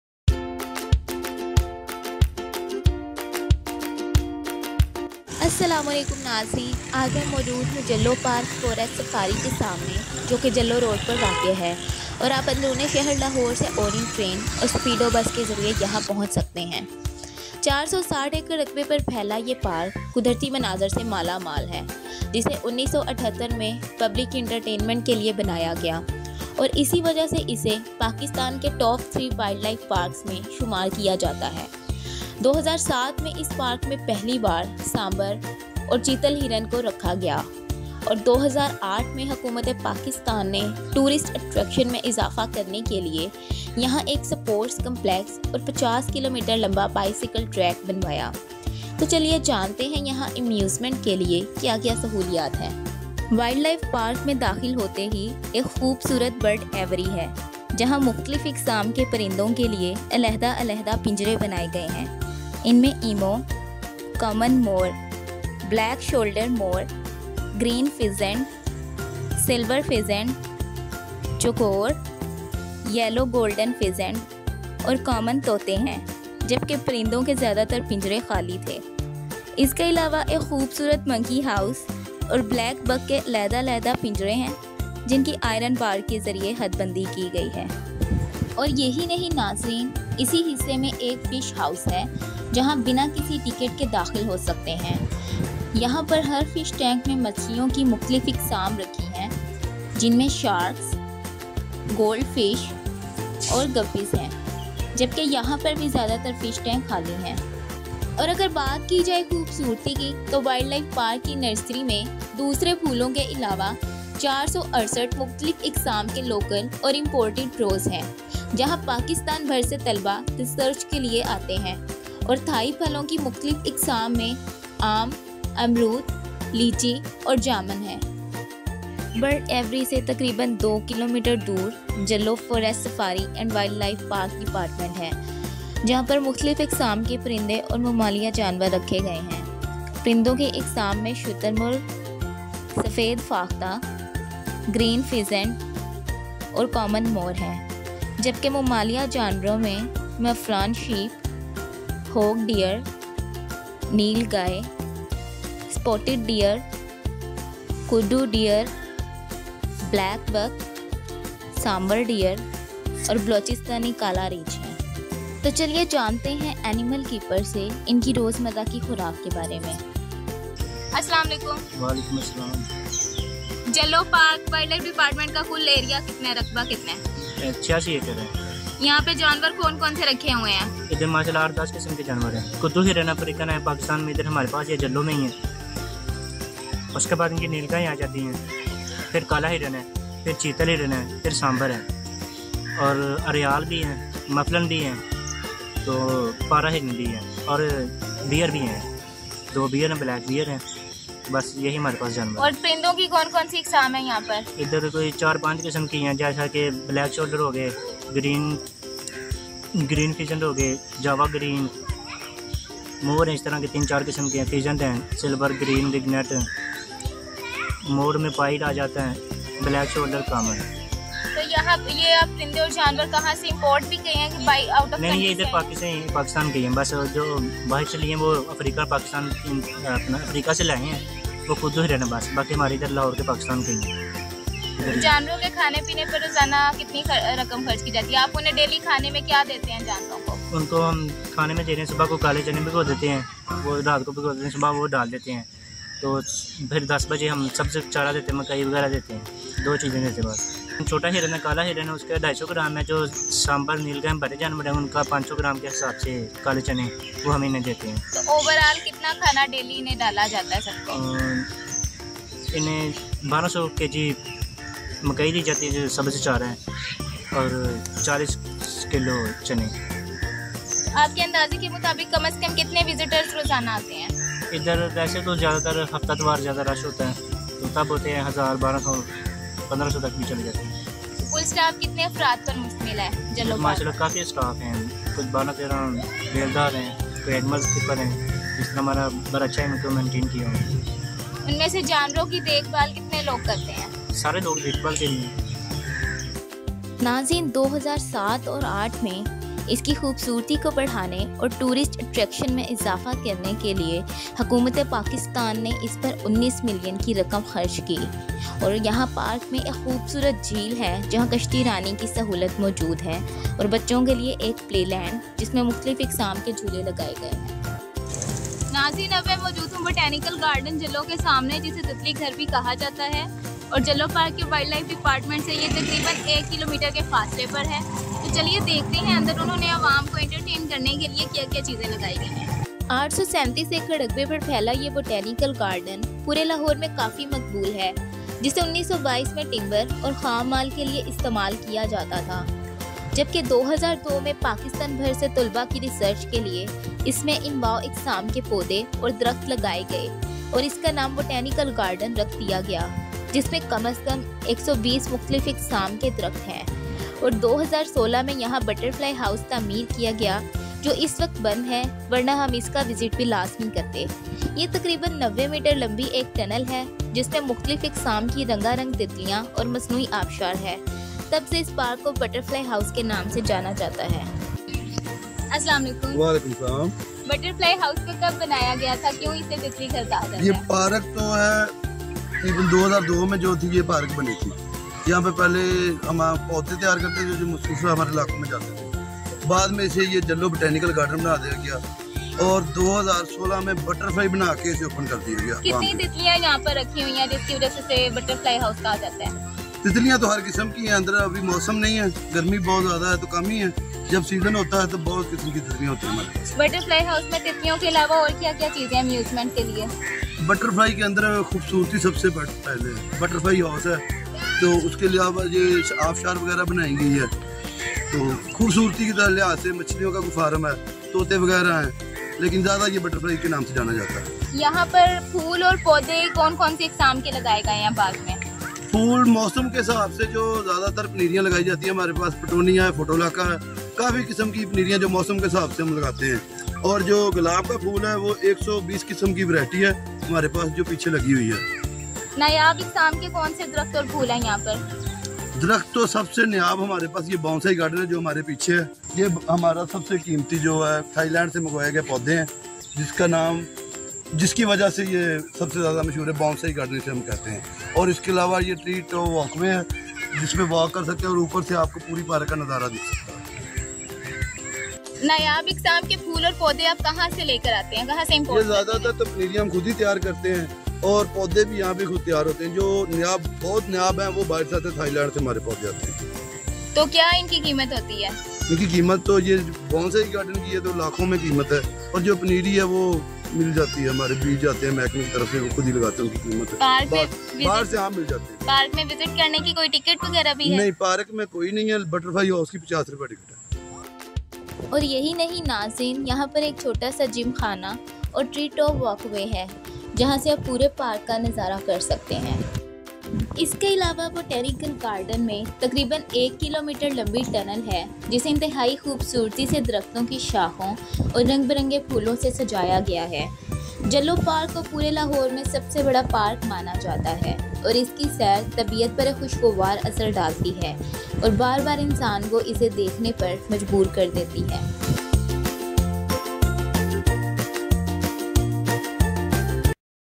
Chalo Chalo Jallo Park Chalo | Come All Let’s Go To Jallo Park
Jallo Park is a complete botanical park in Lahore, even though it takes time to reach there but once you are in Jallo Park you never regret your decision
@dialogueTVPvt. YouTubeChannel